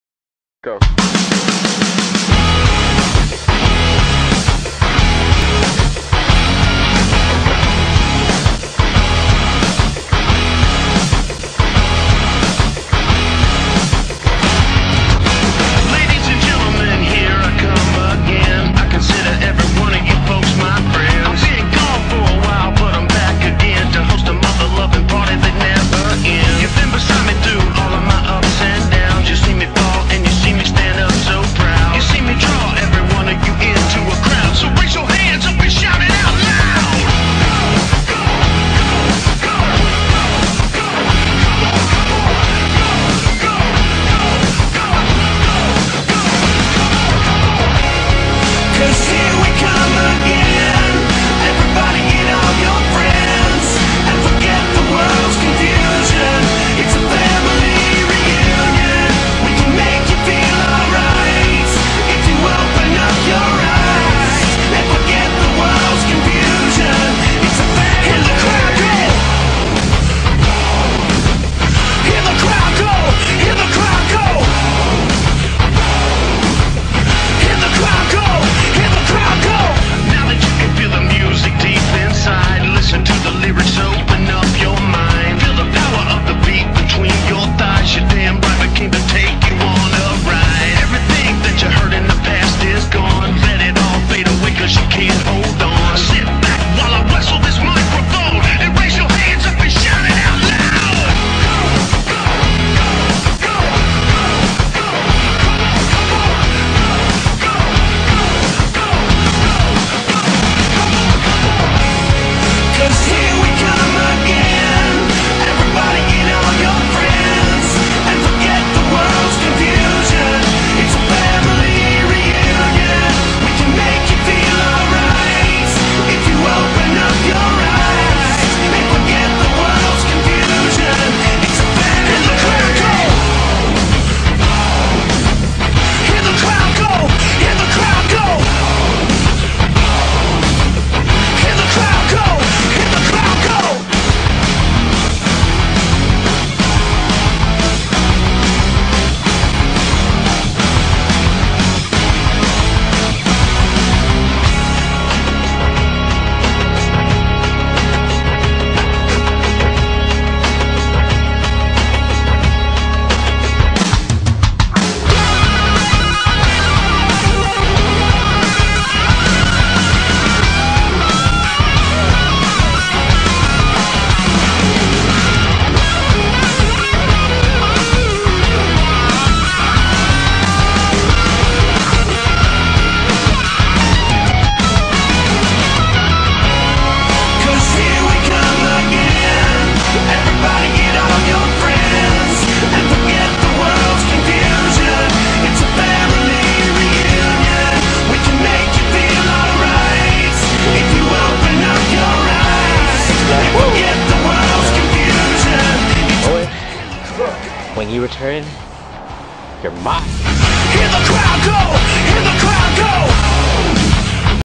New NXW Backyard Wrestling - Family Reunion
The Canadian Backyard Wrestling company, NXW is finally back and ready to deliver more great matches, shows and music videos!

Thank you to everyone who patiently waited, we greatly appreciate it, sorry it took so long!